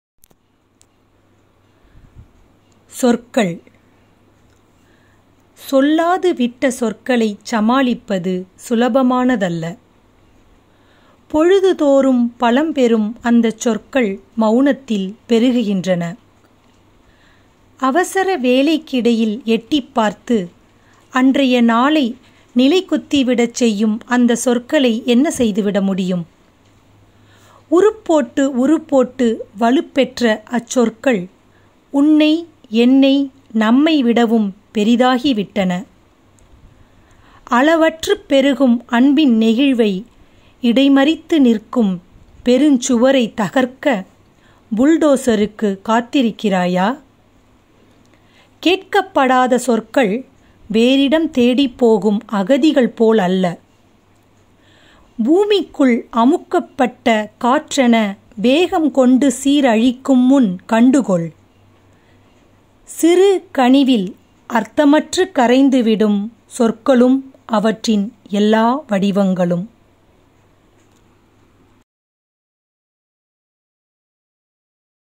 லதா – சொற்கள் – 57 - ஏப்ரல் 2
கவிதையின் தலைப்பு – ‘சொற்கள்’
எழுதியவர் – கவிஞர் லதா
வாசிப்பவர் – திருமதி அழகுநிலா
‘யாருக்கும் இல்லாத பாலை’ கவிதைத் தொகுப்பிலிருந்து
சொல்லாது விட்ட சொற்களைச்
சமாளிப்பது
சுலபமானதல்ல
பொழுதுதோறும் பலம்பெறும்
அந்தச் சொற்கள்
மௌனத்தில் பெருகுகின்றன
அவசர வேலைக்கிடையில்
எட்டிப் பார்த்து
அன்றைய நாளை
நிலைகுத்திவிடச் செய்யும்
அந்தச் சொற்களை என்ன செய்துவிட முடியும்?
உருப்போட்டு உருப்போட்டு
வலுப்பெற்ற அச்சொற்கள்
உன்னை, என்னை
நம்மைவிடவும்
பெரிதாகிவிட்டன.
அளவற்றுப் பெருகும் அன்பின்
நெகிழ்வை
இடைமறித்து நிற்கும்
பெருஞ்சுவரைத் தகர்க்க
புல்டோசருக்குக் காத்திருக்கிறாயா?
கேட்கப்படாத சொற்கள்
வேறிடம் தேடிப் போகும் அகதிகள்
போலல்ல
பூமிக்குள் அமுக்கப்பட்ட காற்றென
வேகங்கொண்டு சீரழிக்கும் முன்
கண்டுகொள்
சிறு கனிவில்
அர்த்தமற்றுக் கலைந்துவிடும்
சொற்களும்
அவற்றின் எல்லா வடிவங்களும்